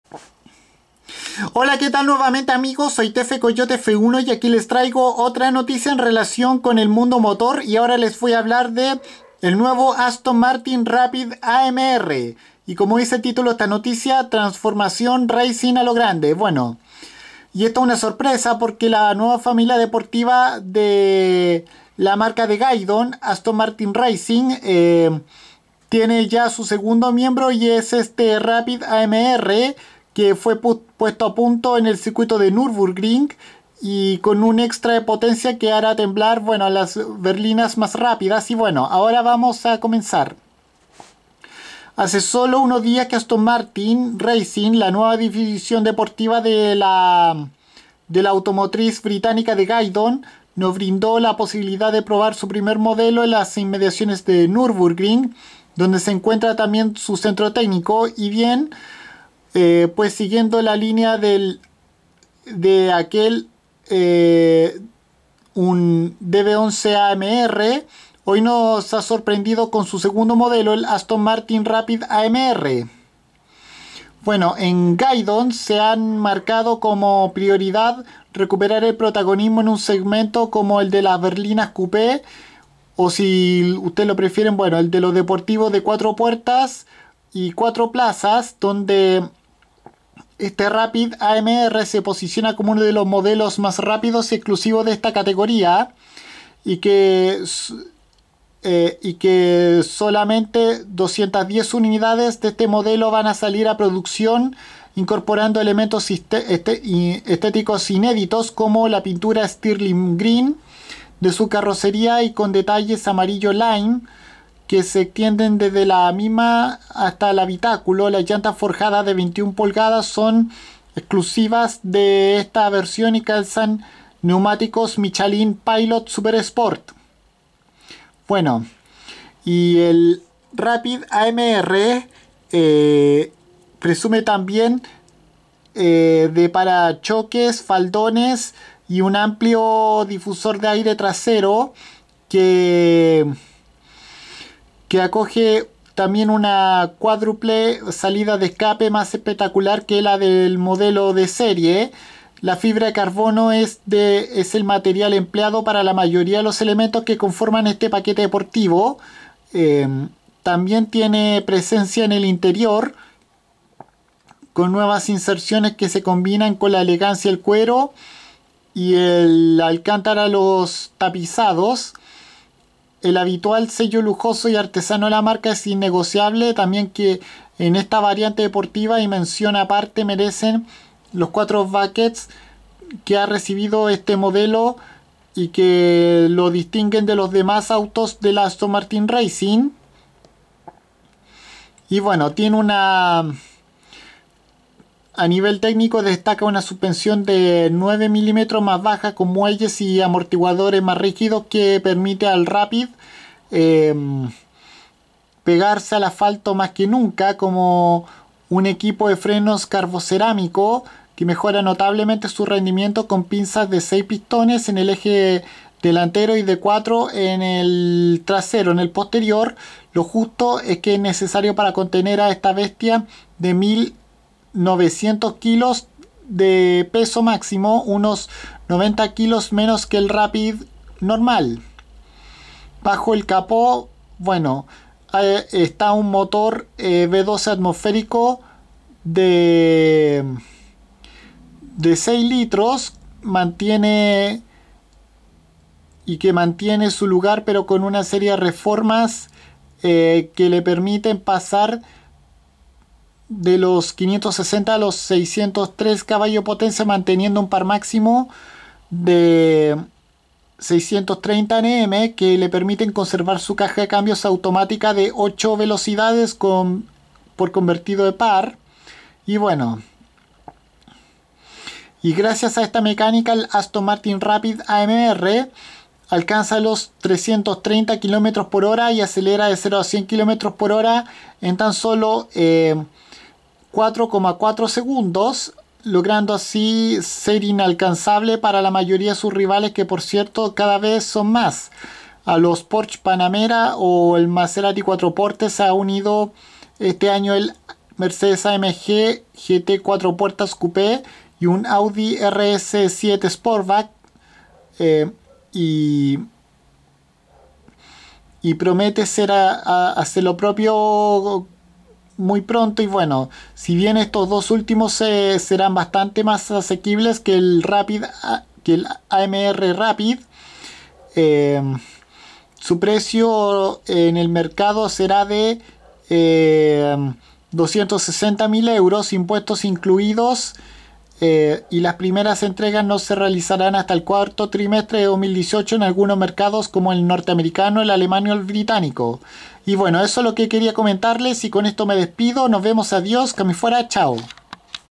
Información sobre el Aston Martin Rapide AMR
El modelo berlina de la automotriz británica de Gaydon recibe una nueva versión deportiva y fue puesta a punto en Nurburgring